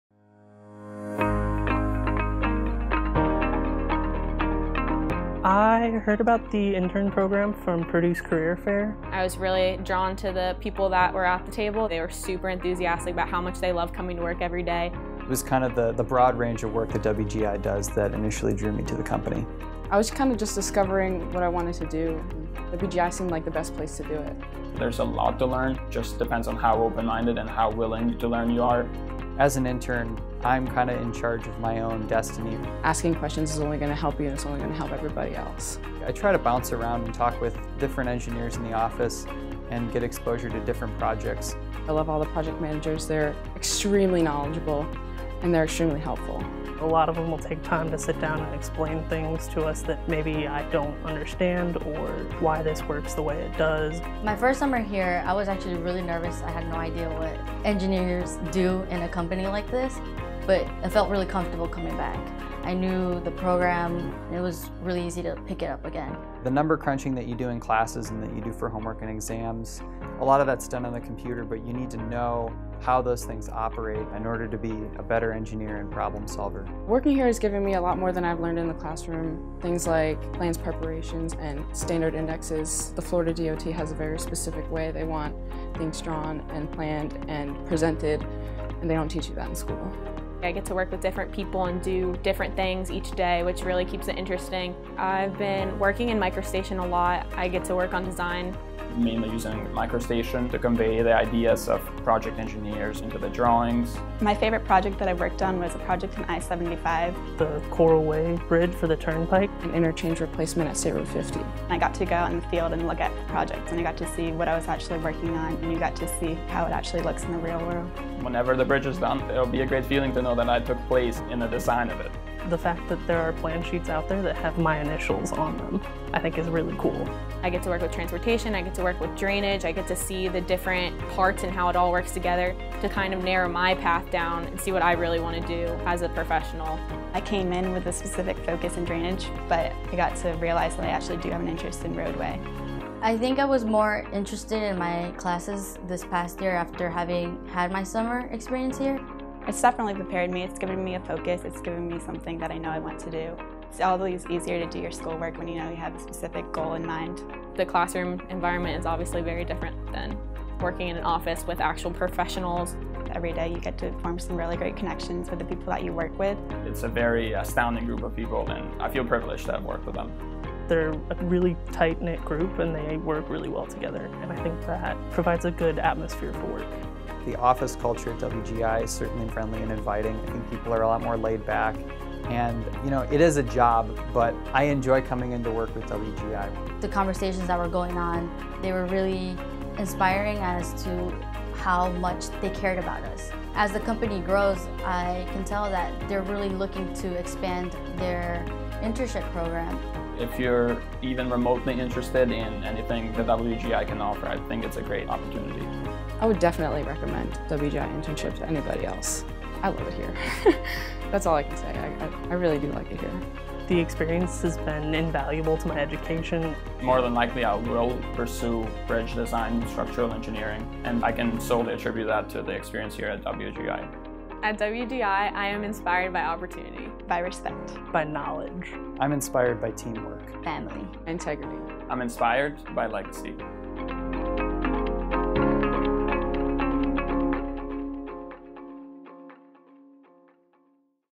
WGI Internship Program 2016
WGI is an avid supporter of providing a real-life experience through  internships, getting students out of the classroom and into the real world of engineering for a true hands-on experience. Listen to our interns talk about the value of their experience at WGI, and contact our Human Resources Department if you think this might be the opportunity for you!